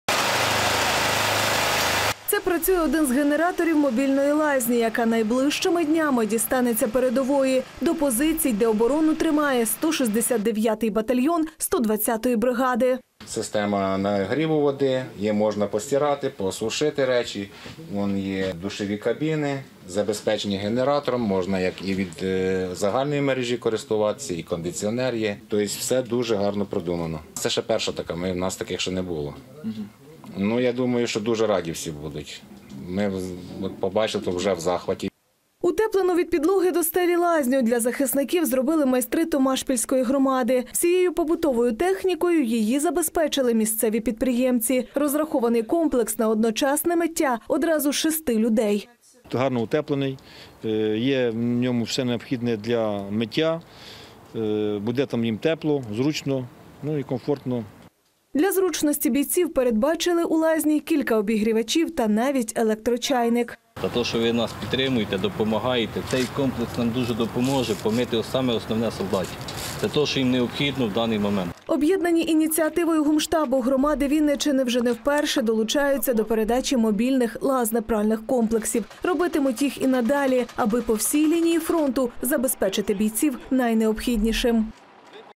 Томашпільська громада передала лазне-пральний комплекс на передову
6 душових кабін, пральна машина, сушарка, бойлери для підігріву води, та обігрівачі - так оснастили лазне-пральний комплекс для військових у Томашпільській громаді. Мобільна лазня здатна працювати при підключенні до електроживлення і навіть без.